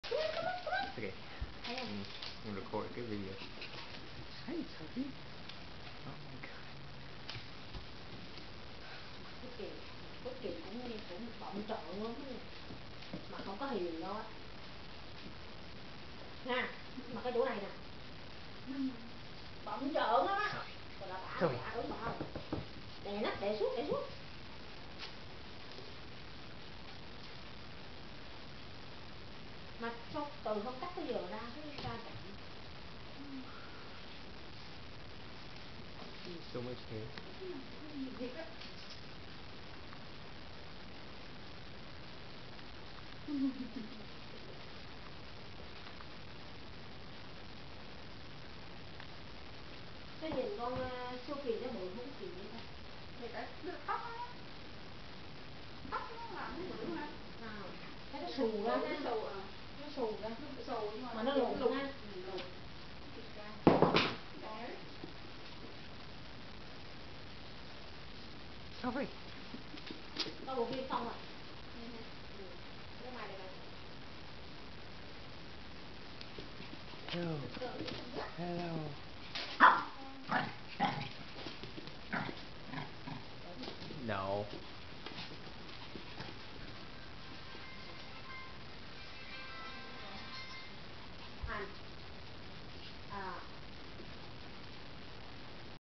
Wookie & Sophie shih tzu/pekingnese mix sisters playing
Sophie's hair is so long!